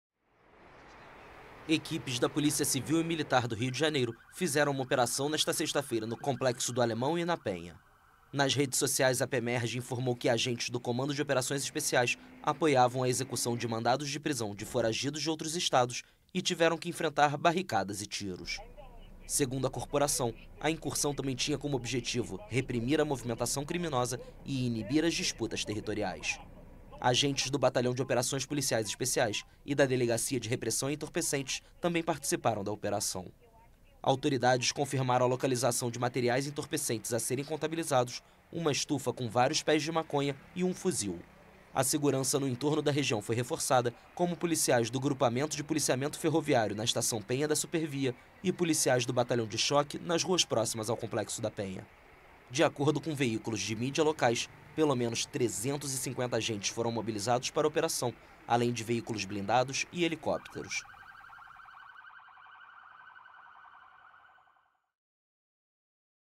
Operação no Complexo do Alemão busca fugitivos de outros estados | AFP
Policiais civis e militares realizaram uma operação nos complexo do Alemão e da Penha, onde foram recebidos com tiros e barricadas, nesta sexta-feira. De acordo com as autoridades, o objetivo era encontrar foragidos de outros estados que buscam abrigo no Rio de Janeiro.